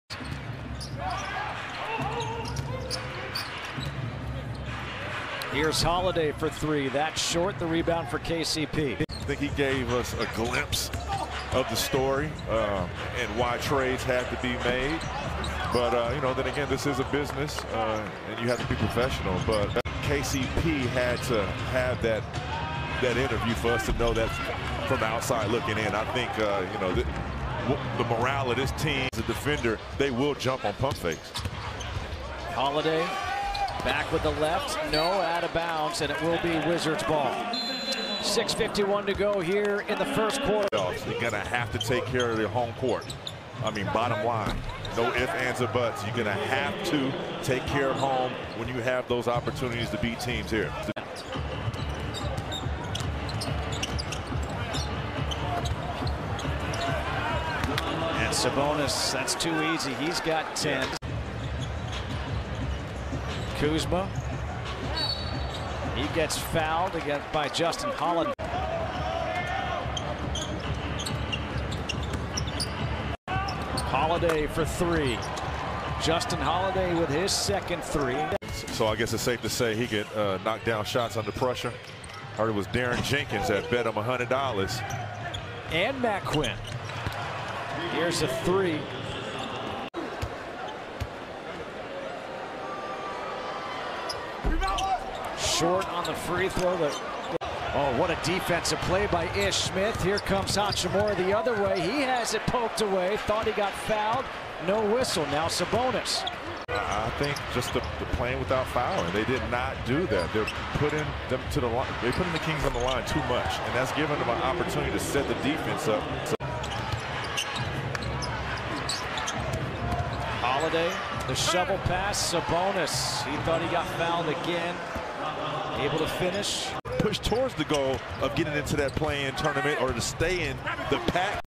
Justin Holiday 12 PTS: All Possessions (2022-02-12)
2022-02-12 Washington Wizards vs Sacramento Kings: Player Highlights Justin Holiday
    12 PTS, 2 REB, 2 AST, 1 STL.
        4 of 6 from 3.
        4 of 8 FGs in 29:51 minutes.